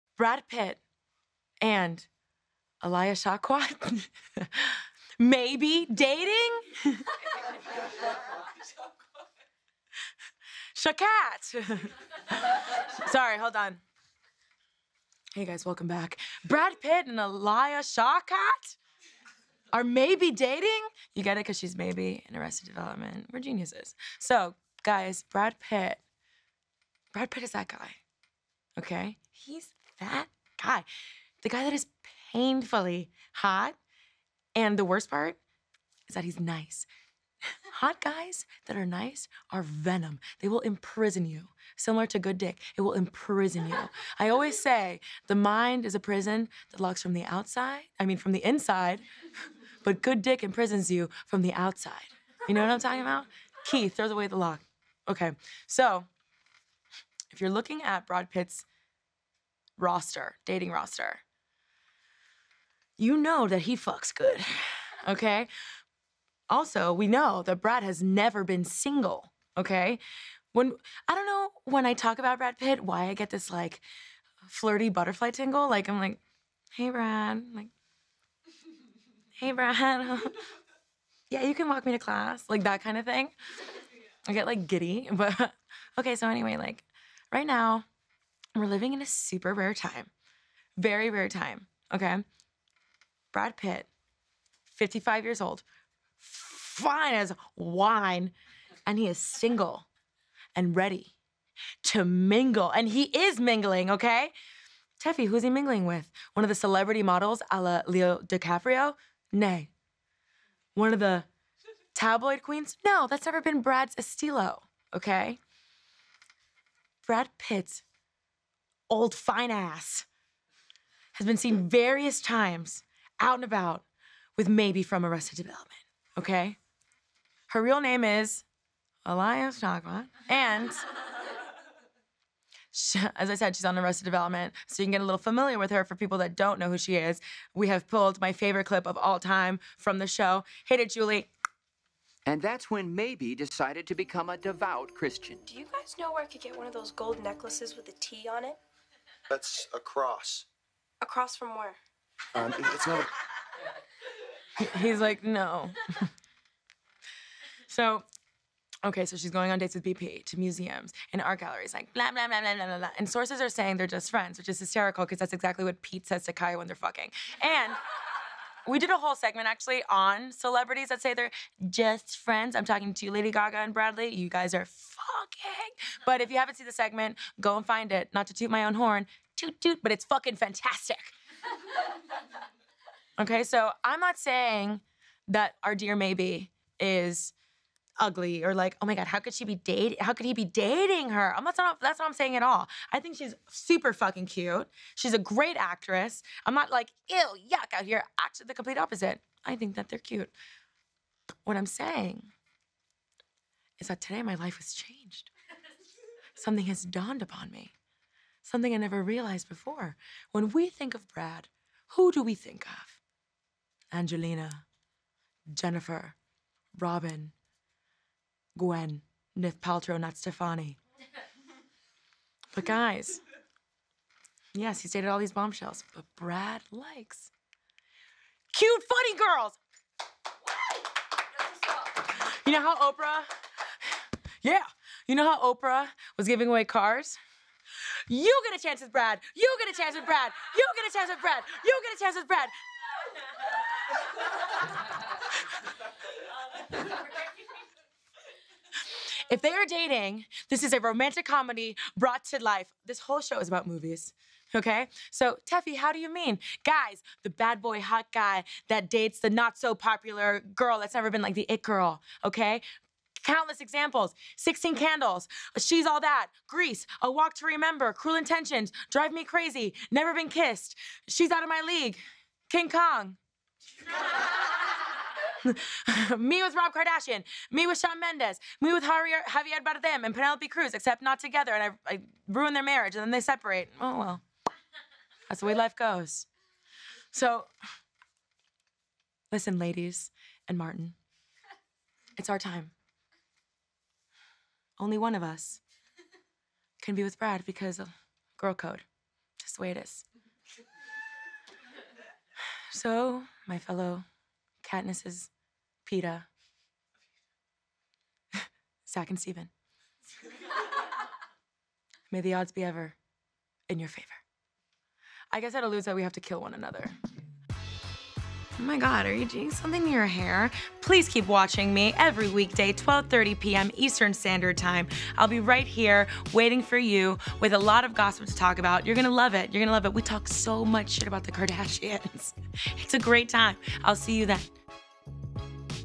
Brad Pitt and Alia Shawkat are MAYBE dating!
Watch Tefi live weekdays at 12:30pm ET.

Dreams is a live TV network for the internet.